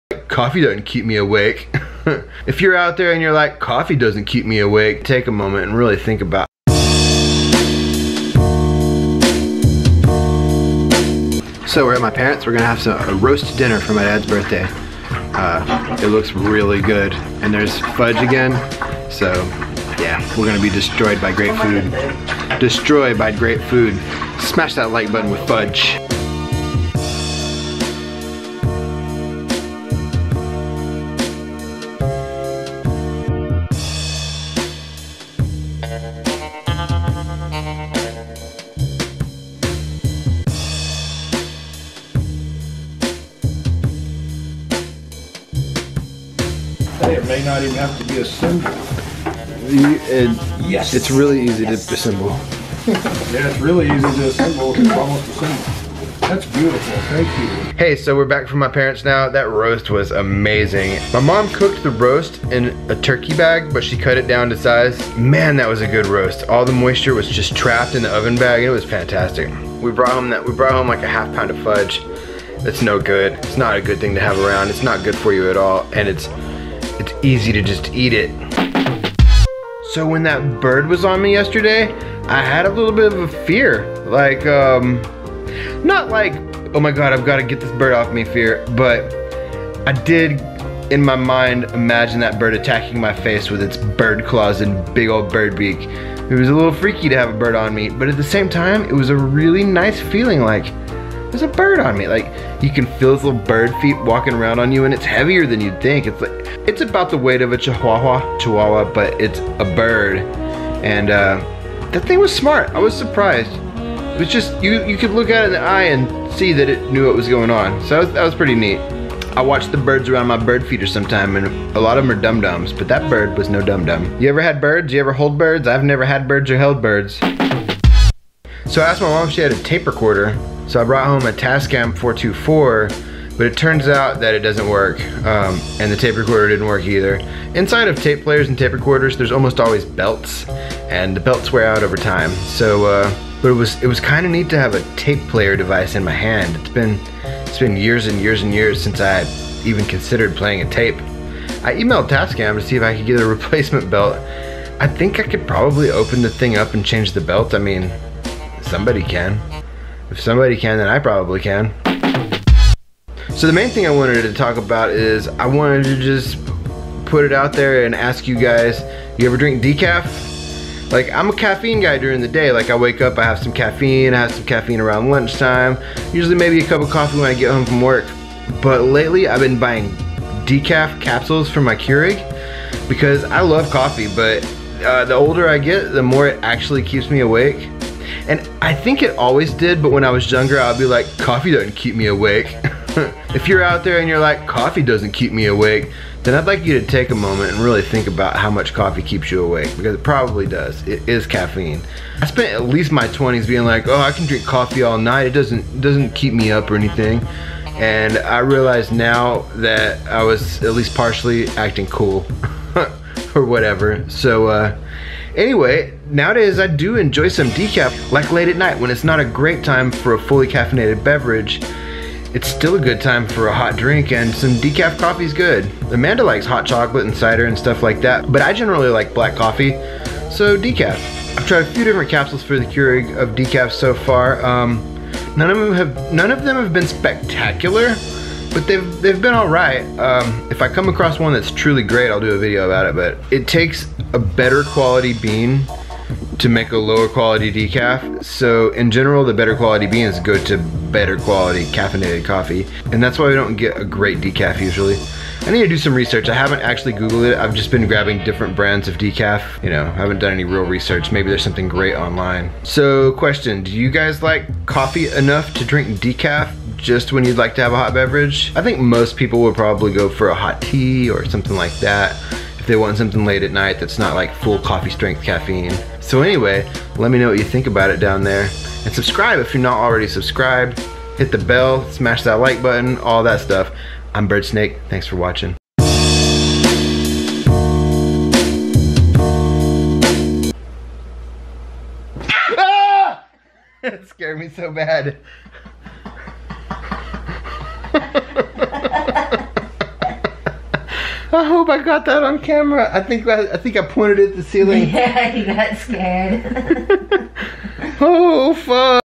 Too much coffee... can't sleep
These days when I have too much coffee I can't sleep. Also, we go to my parent's house for my dad's birthday and also I talk about decaf and birds.